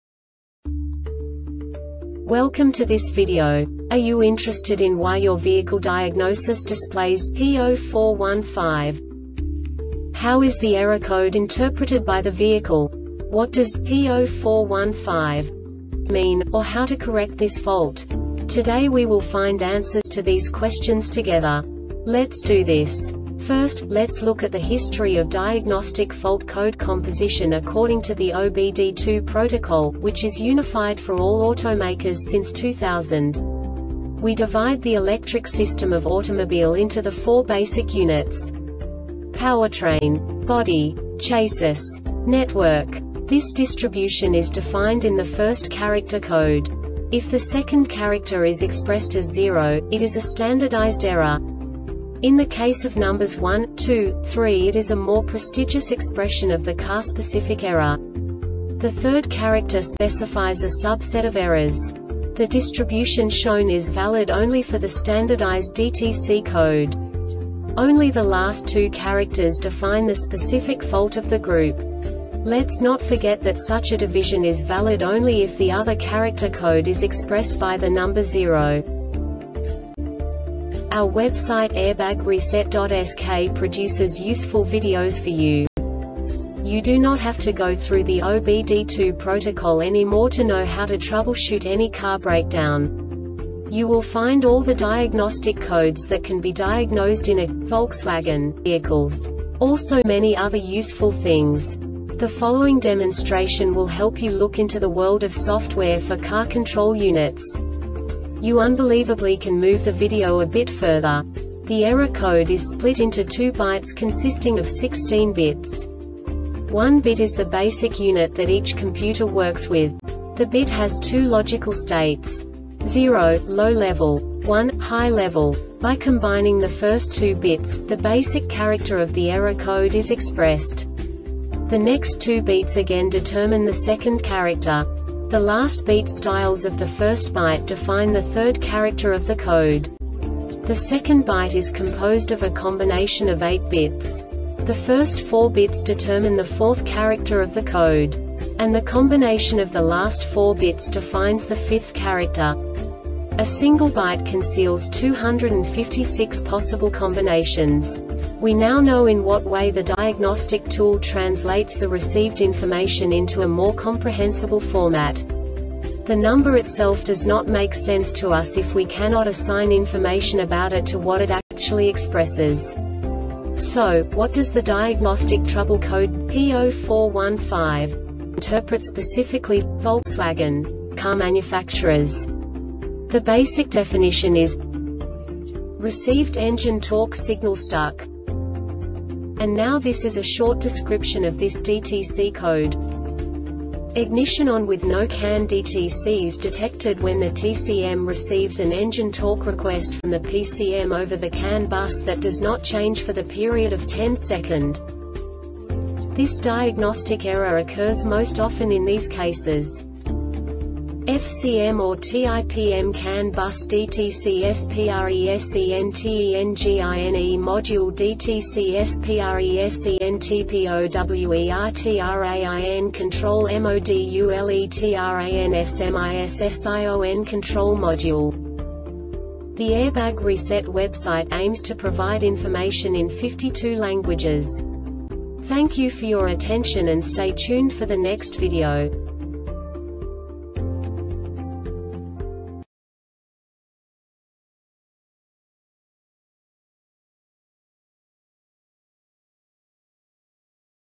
DTC Volkswagen P0415 Short Explanation
Contents:
0:21 Basic DTC analysis according to OBD2 protocol standard.
1:48 Insight into programming
2:58 A diagnostically understandable description of the Diagnostic Trouble Code
3:05 Basic error definition

Make:
Volkswagen
Code:
P0415
Definition:
RECEIVED ENGINE TORQUE SIGNAL STUCK
Description:
Ignition on with no CAN DTCs detected. When the TCM receives an engine torque request from the PCM over the CAN Bus that does not change for the period of 1.0 second
Cause:
FCM or TIPM CAN BUS DTCS PRESENTENGINE MODULE DTCS PRESENTPOWERTRAIN CONTROL MODULETRANSMISSION CONTROL MODULE